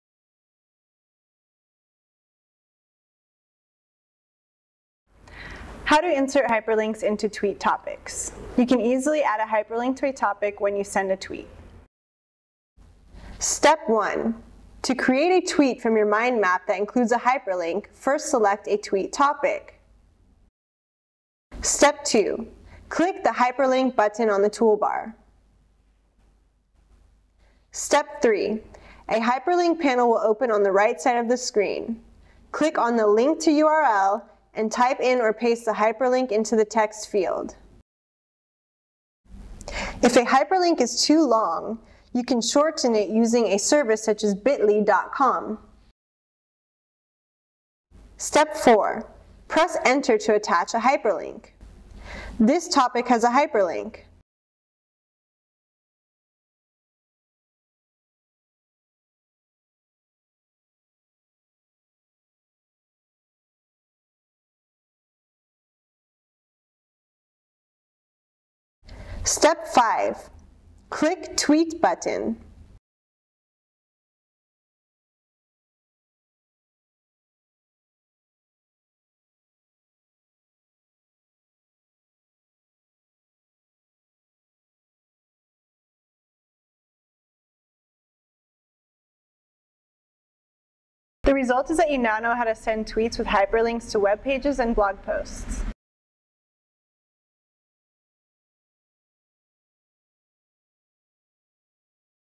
How To Insert Hyperlinks Into Tweet Topics
Demonstration on how to insert hyperlinks into Tweet topics